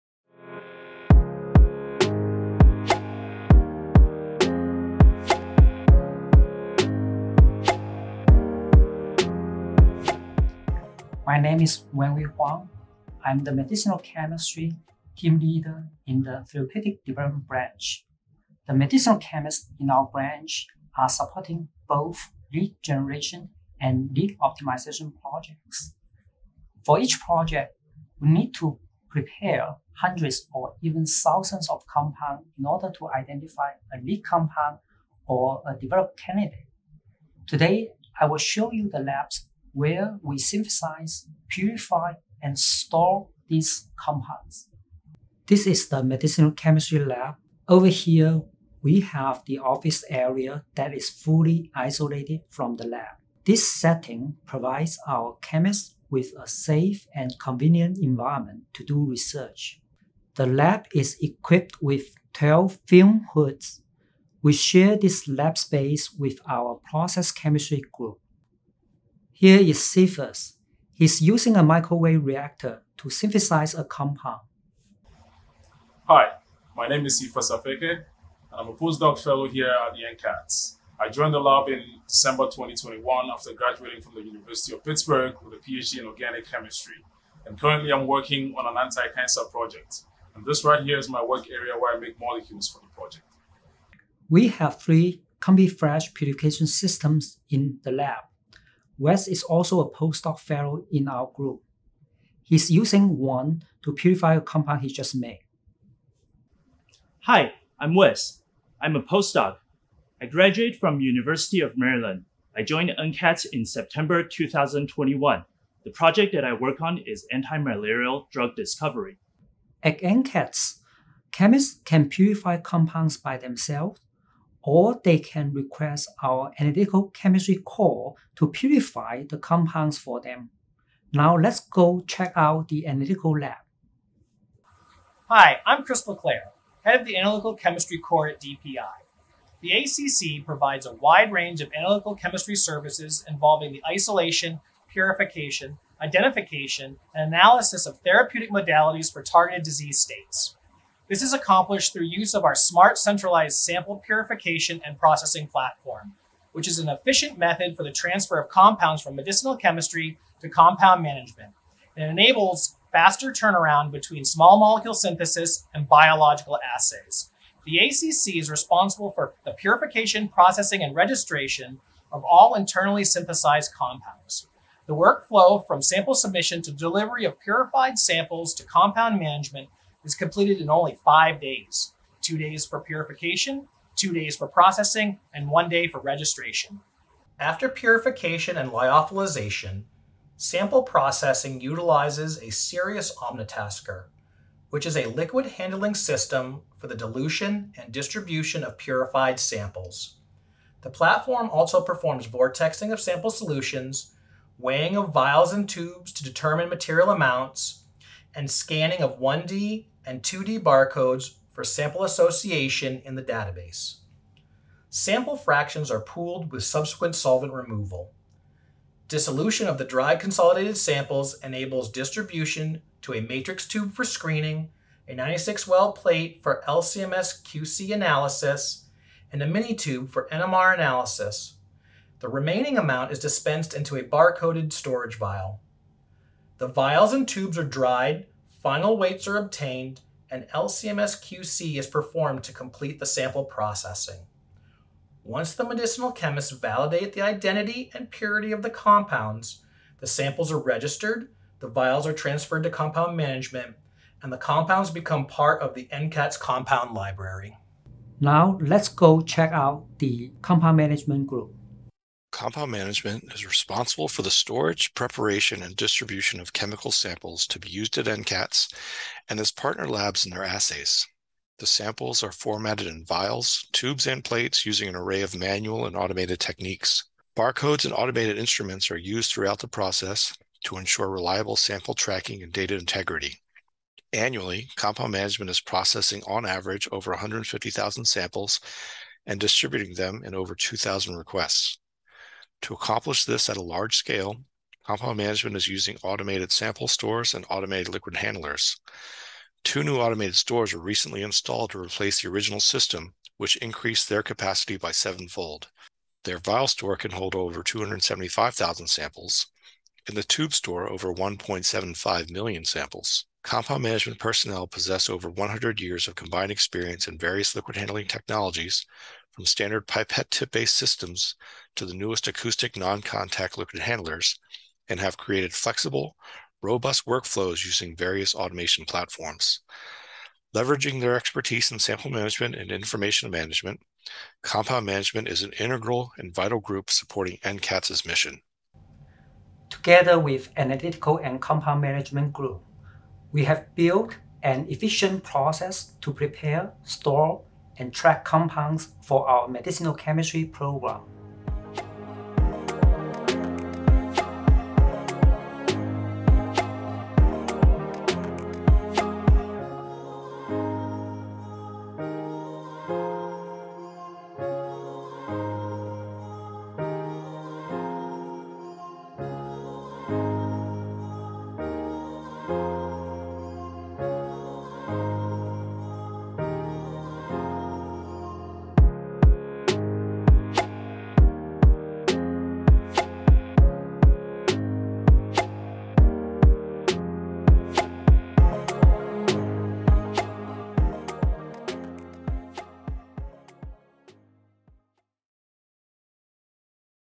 Lab tour — TDB Medicinal Chemistry Program, NCATS
Step inside the walls of NCATS Medicinal Chemistry lab in the Therapeutic Development Branch to learn about how we synthesize, purify and store compounds in the drug discovery process.